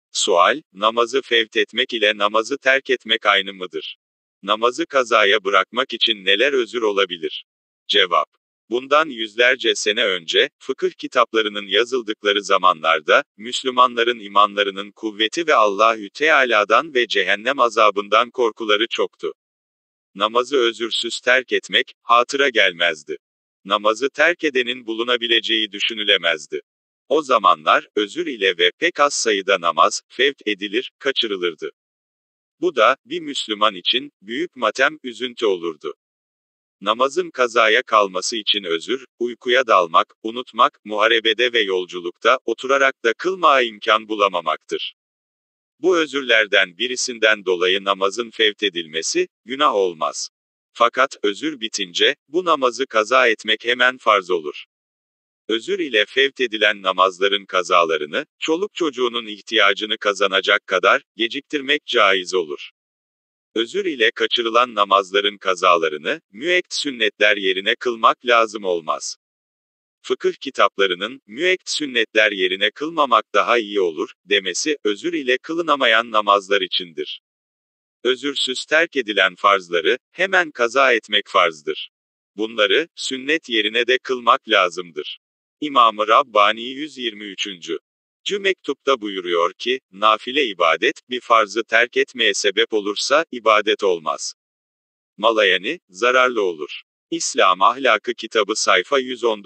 Namazı terketmek ve fevt etmek - İslam Ahlakı
Sual: Namazı fevt etmek ile namazı terketmek aynı mıdır? Namazı kazaya bırakmak için neler özür olabilir?
Cevap: Bundan yüzlerce sene önce, fıkıh kitaplarının yazıldıkları zamanlarda, Müslümanların imanlarının kuvveti ve Allahü teâlâdan ve Cehennem azabından korkuları çoktu. Namazı özürsüz terk etmek, hatıra gelmezdi. Namazı terk edenin bulunabileceği düşünülemezdi. O zamanlar, özür ile ve pek az sayıda namaz, (Fevt) edilir, kaçırılırdı. Bu da, bir Müslüman için, büyük matem, üzüntü olurdu. Namazın kazaya kalması için özür, uykuya dalmak, unutmak, muharebede ve yolculukta, oturarak da kılmağa imkân bulamamaktır. Bu özürlerden birisinden dolayı namazın fevt edilmesi, günah olmaz. Fakat, özür bitince, bu namazı kaza etmek hemen farz olur. Özür ile fevt edilen namazların kazalarını, çoluk çocuğunun ihtiyacını kazanacak kadar, geciktirmek câiz olur. Özür ile kaçırılan namazların kazalarını, müekked sünnetler yerine kılmak lâzım olmaz. Fıkıh kitaplarının (müekked sünnetler yerine kılmamak daha iyi olur) demesi, özür ile kılınamayan namazlar içindir. Özürsüz terk edilen farzları, hemen kaza etmek farzdır. Bunları, sünnet yerine de kılmak lâzımdır. İmâmı Rabbânî 123. cü mektupta buyuruyor ki, (Nafile ibadet, bir farzı terk etmeğe sebep olursa, ibadet olmaz. Malayani, zararlı olur.)
(İslâm Ahlâkı Kitabı sayfa 119)